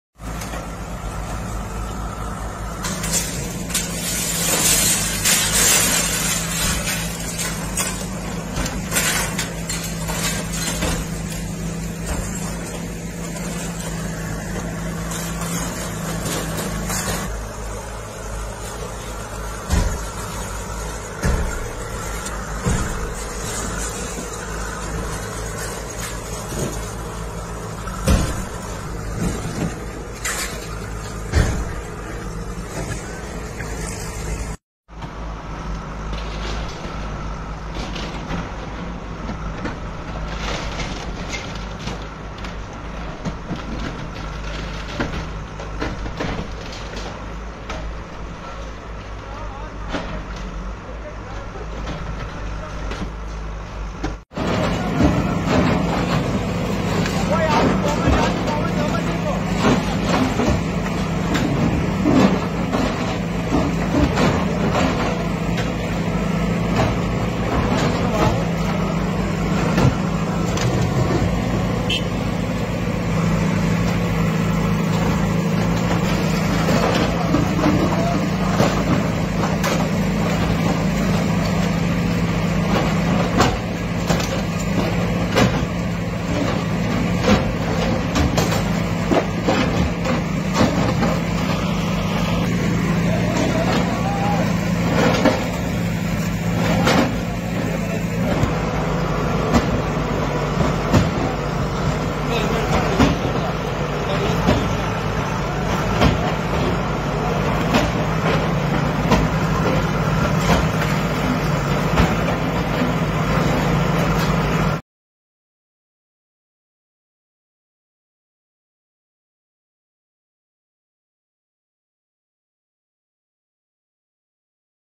JLKMEDIA Encroachment Drive starts today at Semthan Bijbehara
JLKMEDIA Encroachment Drive starts today at Semthan Bijbehara in the presence of SDM and Tehsildar Bijbehara.